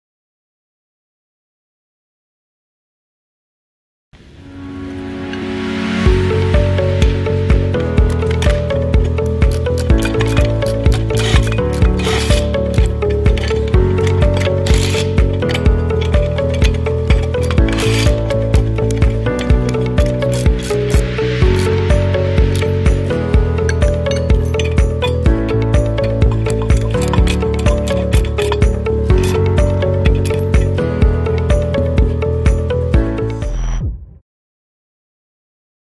Fastening Bolts: Hilti SIW 9-A22 High Torque Impact Wrench
Do you need increased speed for fastening or loosening bolts?

Hilti shares tips and hints on you can save more time on fastening and loosening bolts with the SIW 9-A22 High Torque Impact Wrench, compared to the traditional method of doing things

Interested in learning more?